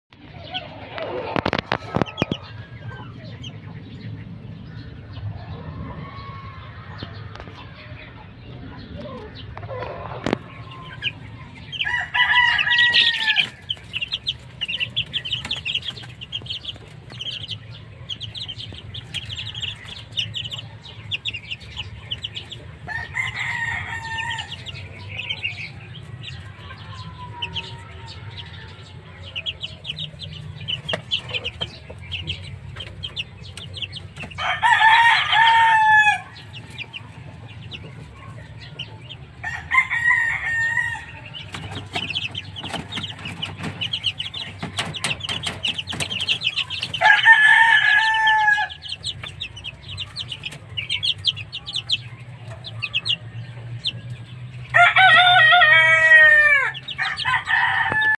Update lang ulit sa aking SiSiW guys Pang 25 days old na,
25 days old na Sisiw